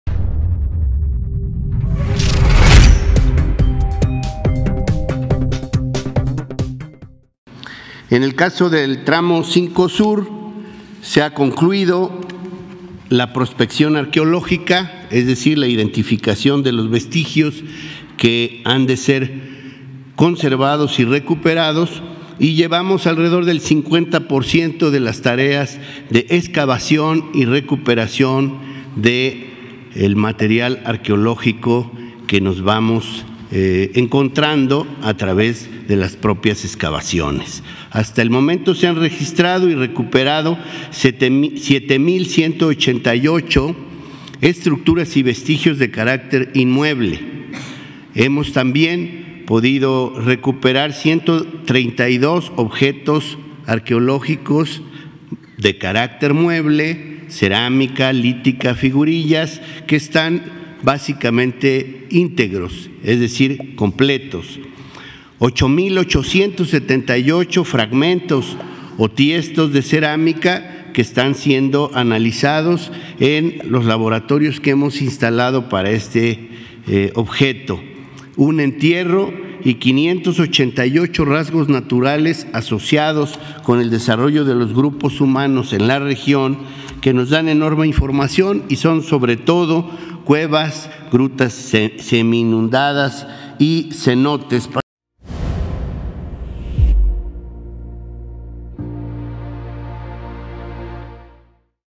MILES DE VESTIGIOS MAYAS SE HAN RECUPERADO EN EL TRAMO 5 SUR DEL TREN MAYA
En la zona comprendida entre Playa del Carmen y Tulum se han recuperado 132 objetos arqueológicos de carácter mueble, como cerámica, lítica, figurillas, las cuales están prácticamente íntegras. Asimismo, han encontrado 8 mil 878 fragmentos de vasijas y materiales que están siendo analizados en los laboratorios que se han instalado para este objeto.